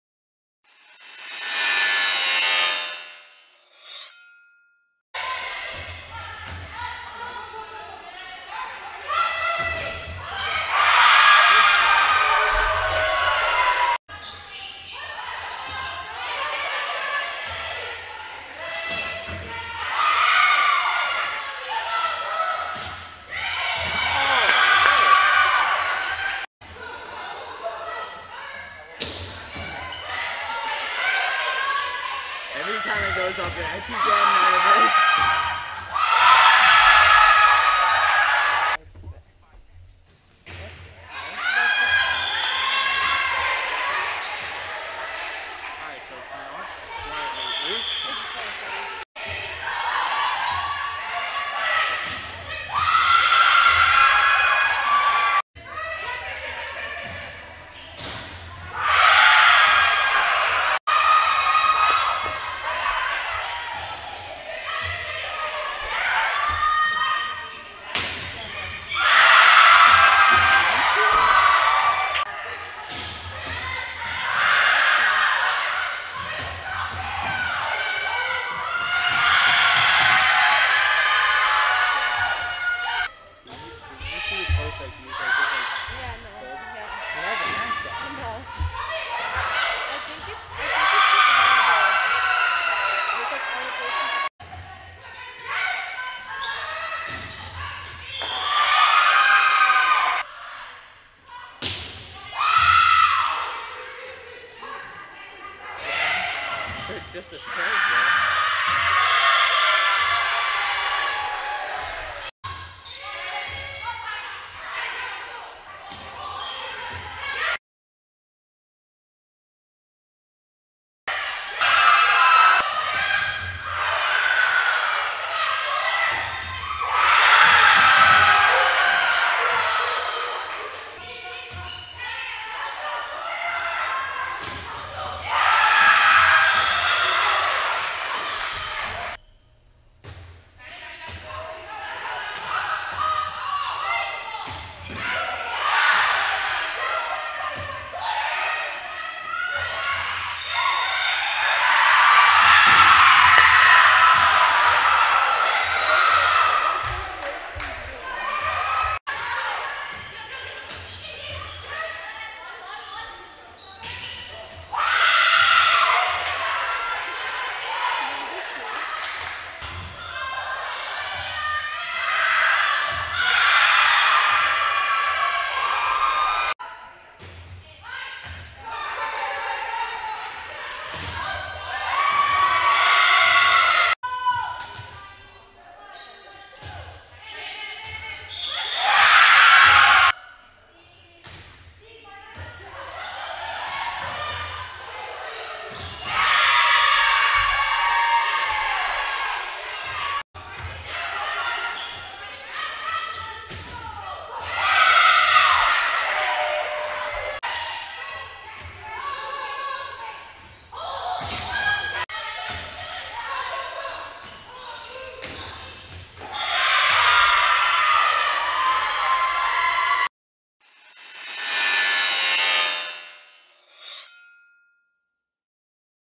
WVB: Merrimack College Volleyball Highlights vs. FDU (3-12-21)
The Merrimack College women's volleyball team played in its first two home matches against Fairleigh Dickinson. The Warriors won their first set of the season in the second match, but FDU won both contests, 3-0 and 3-1. Sophomore libero Taelyr Czech enjoyed a strong day with 38 digs over the two matches.